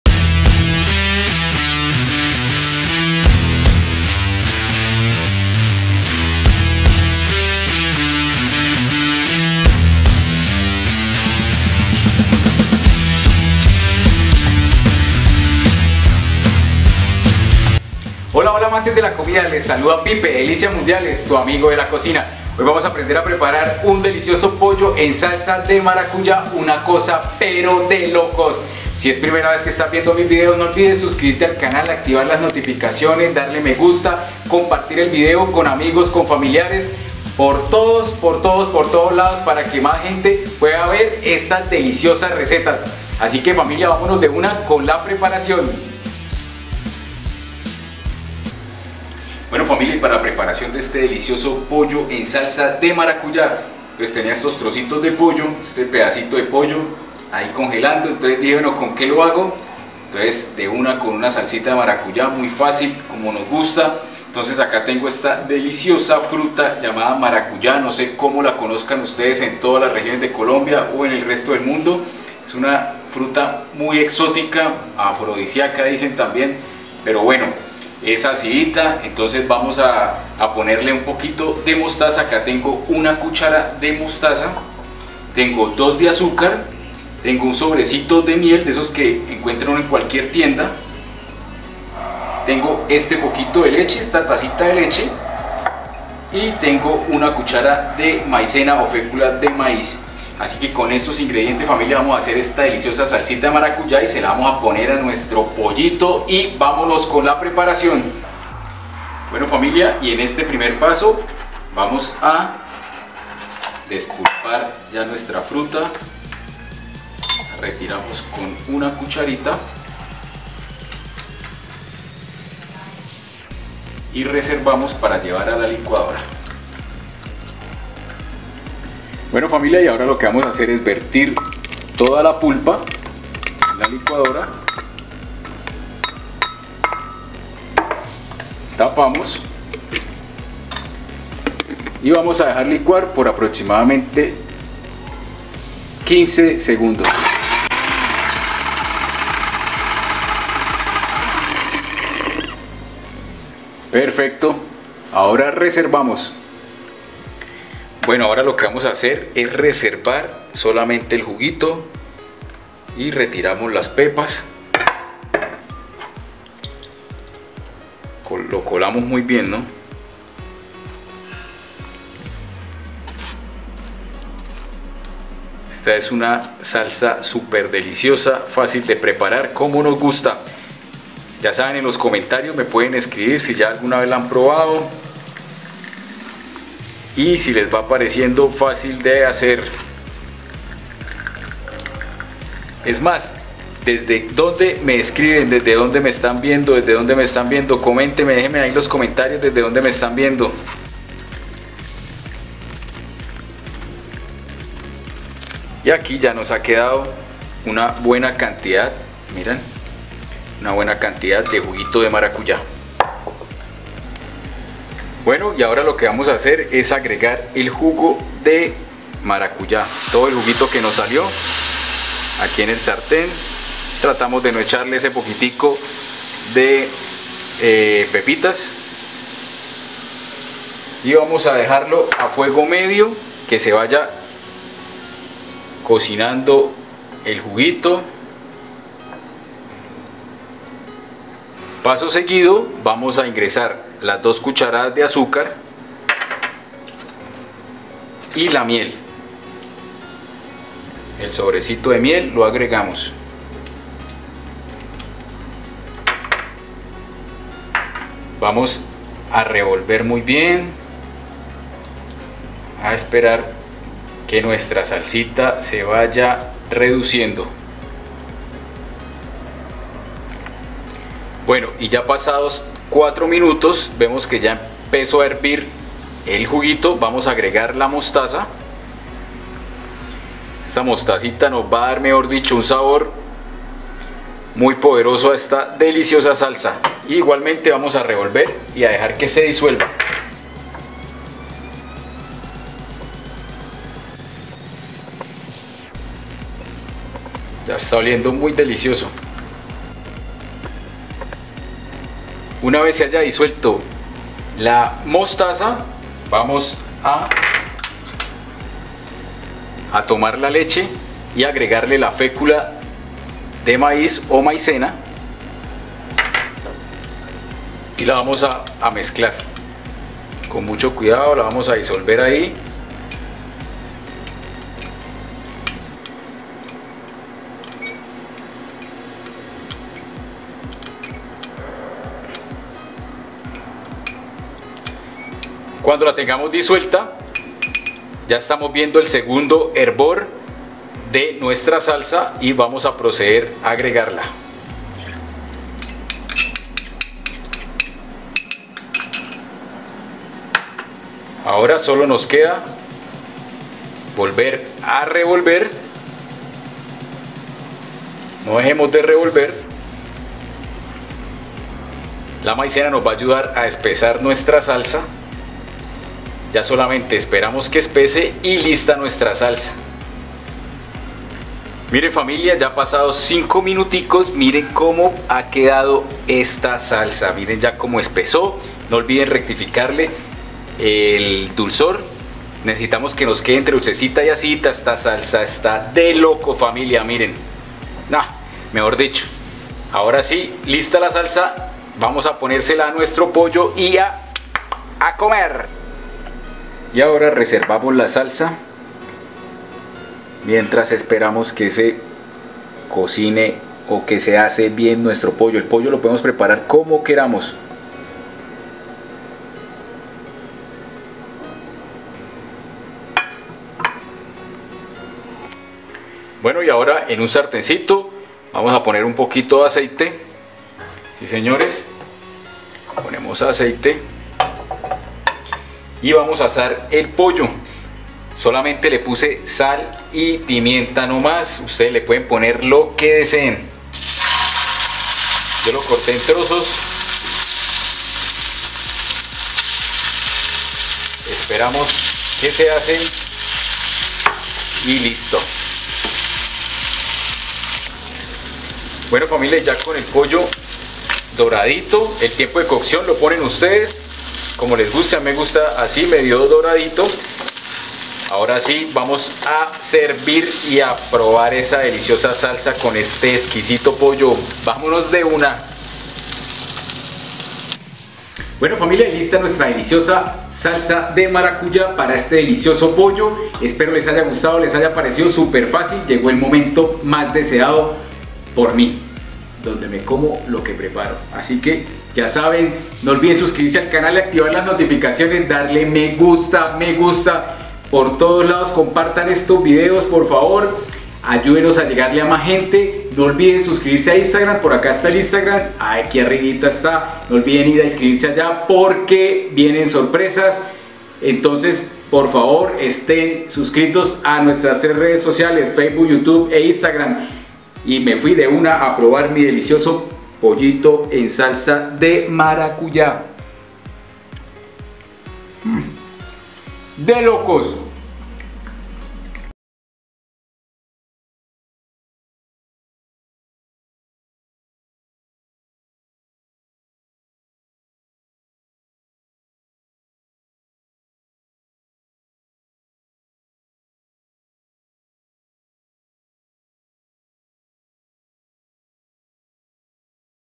Pollo en salsa de maracuya - receta exótica
En el capítulo de hoy aprenderemos a preparar un delicioso pollo exótico en salsa de maracuya sigue el paso a paso y te quedará una cosadelocos